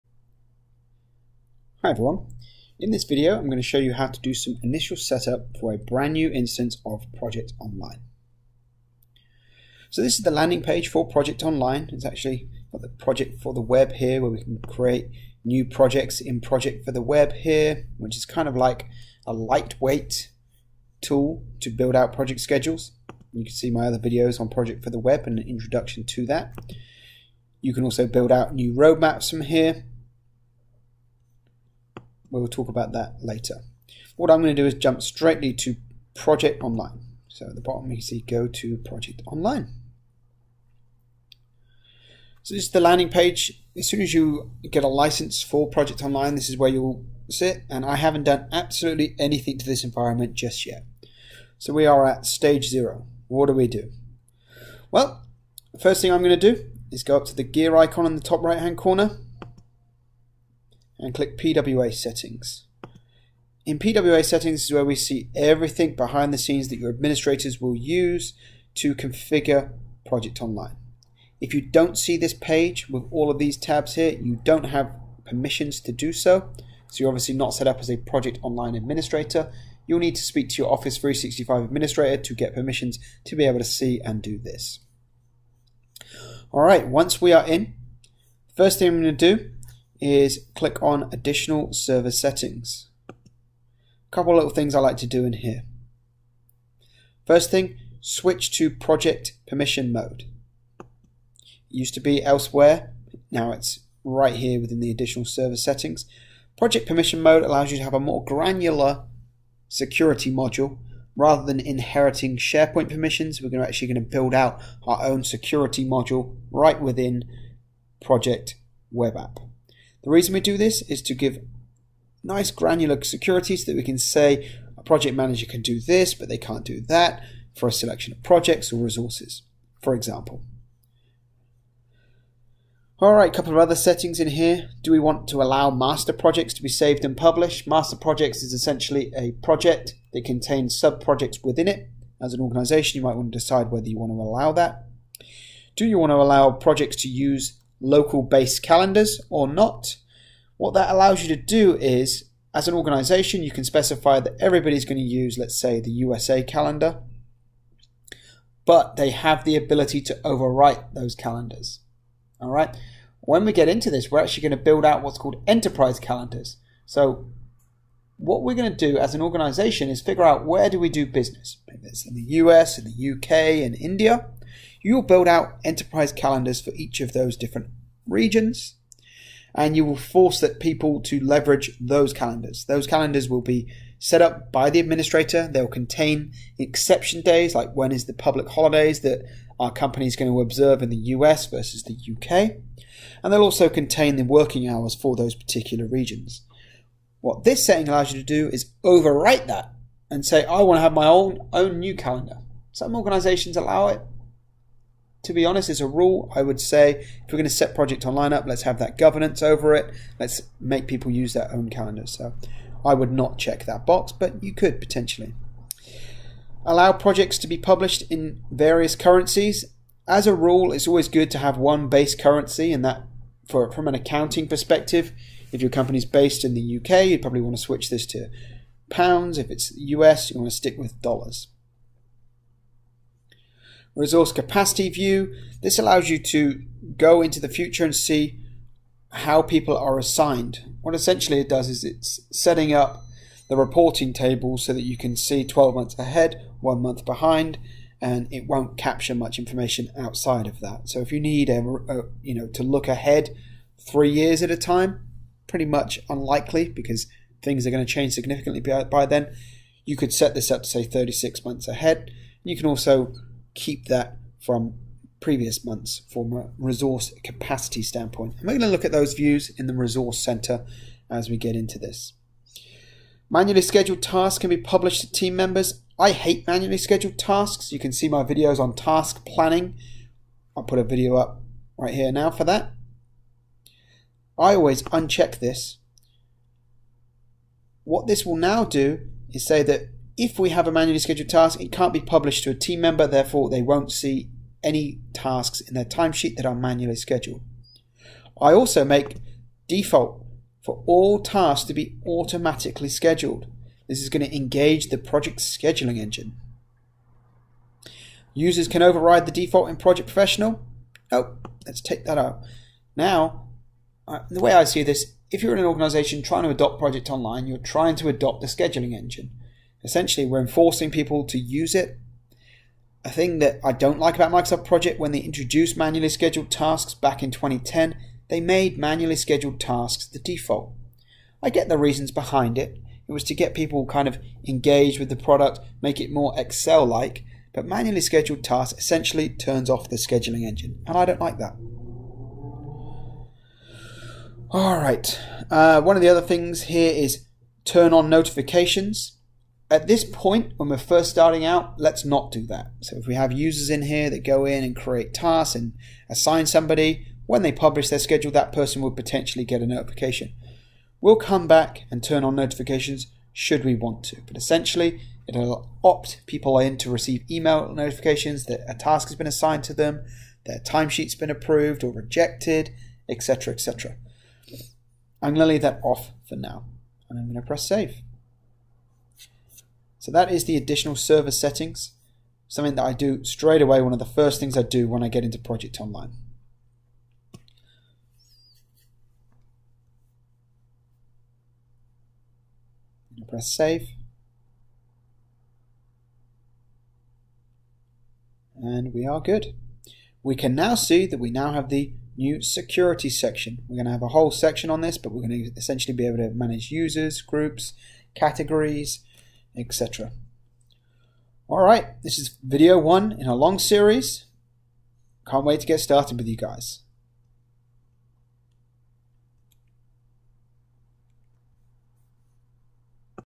Getting Started with Project Online
In this video I cover a few basic settings to help you get started with configuring Project Online.  This is the first in a series of videos.